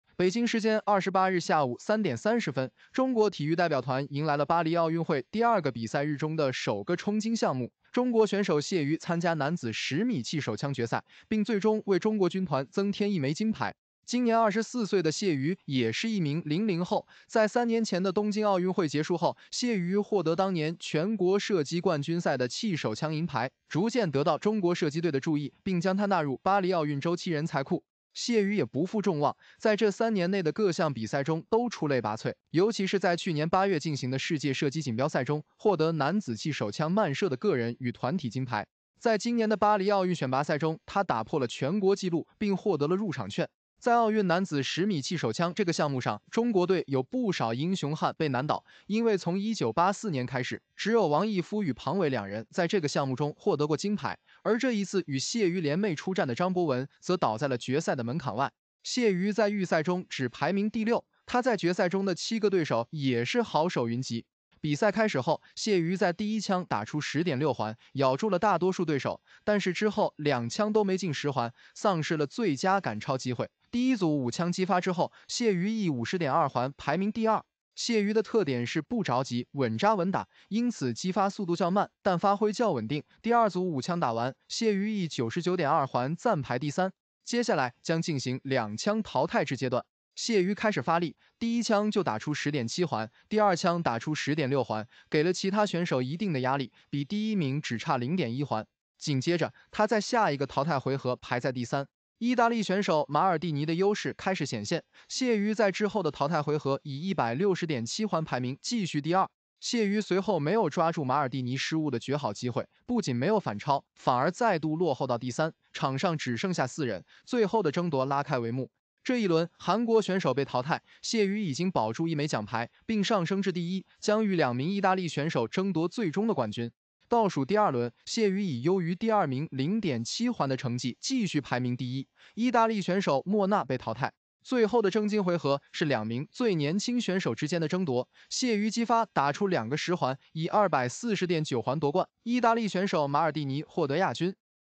第三金！谢瑜在巴黎奥运会男子十米气手枪决赛获胜，为中国再添一金
第三金！谢瑜在男子十米气手枪决赛获胜，为中国再添一金